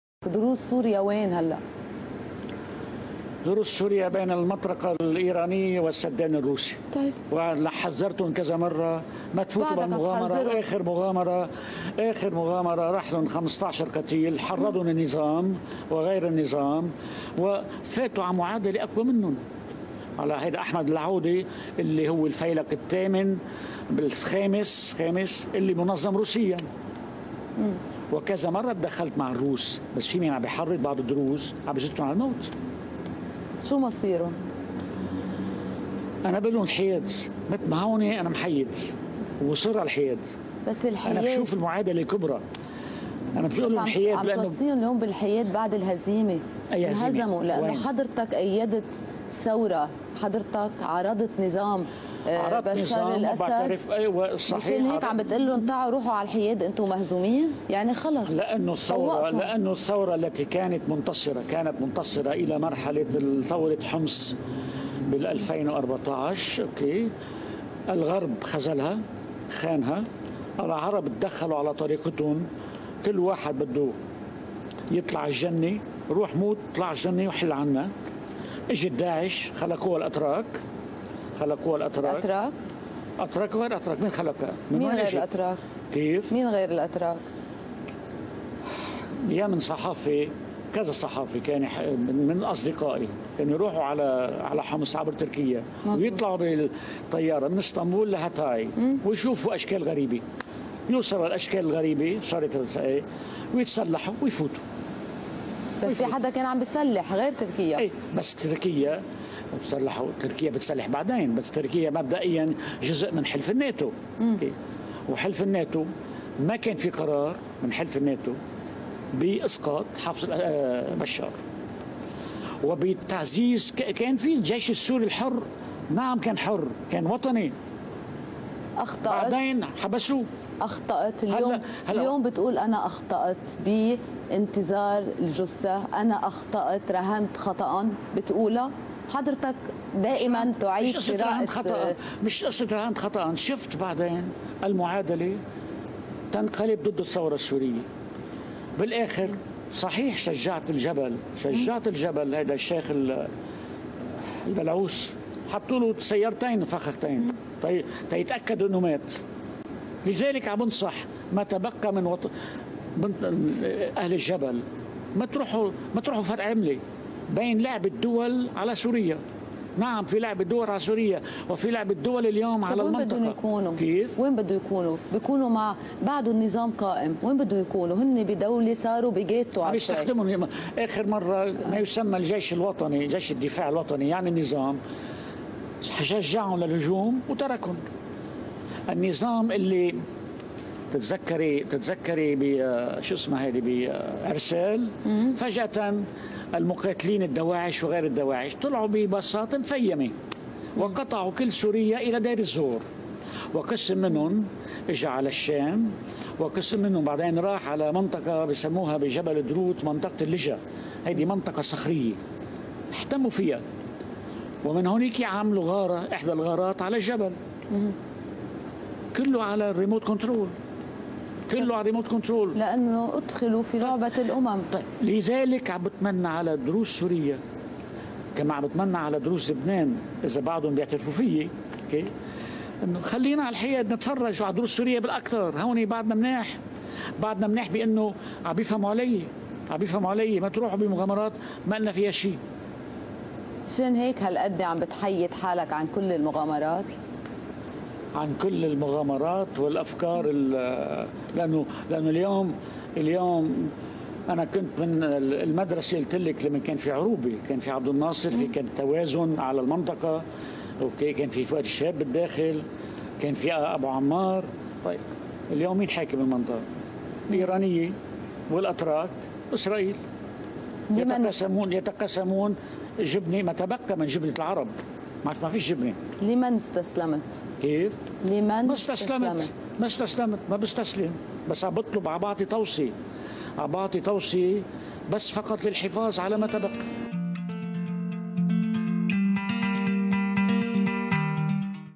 وليد جنبلاط يوجّه رسالة إلى أهل الجبل : "ما تروحوا فرق عملة".. ما القصة؟ Jumblatt
وجّه رئيس التقدمي الاشتراكي، وليد جنبلاط،  رسالةً لـ أهل الجبل، عبر قناة الجديد، قائلًا: "انتبهو ما تروحوا فرق عملة".